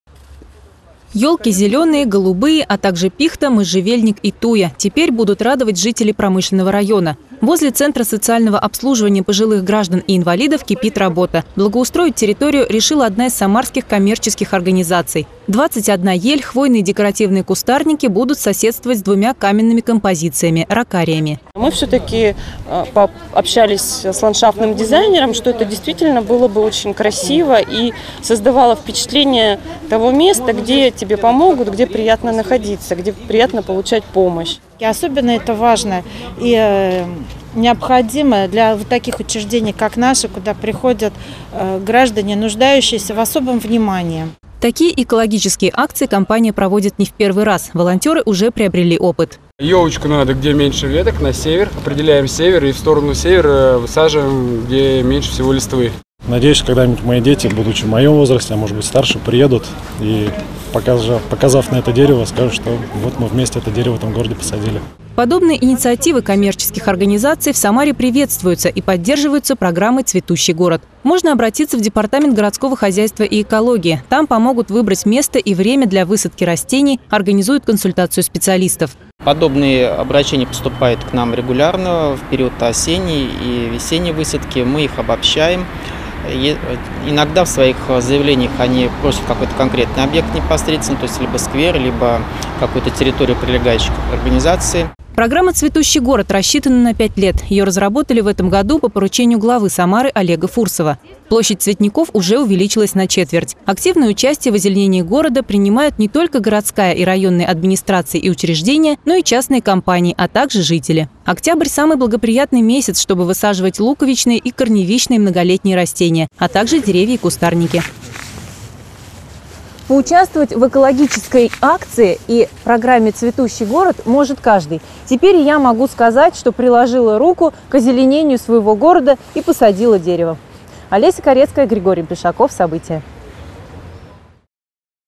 В Самаре продолжается программа по озеленению
Цветущий город даже осенью – в Самаре продолжается программа по озеленению, активное участие принимают не только жители, но и целые организации. В Центре социального обслуживания пожилых граждан и инвалидов высадили ели и декоративные кустарники.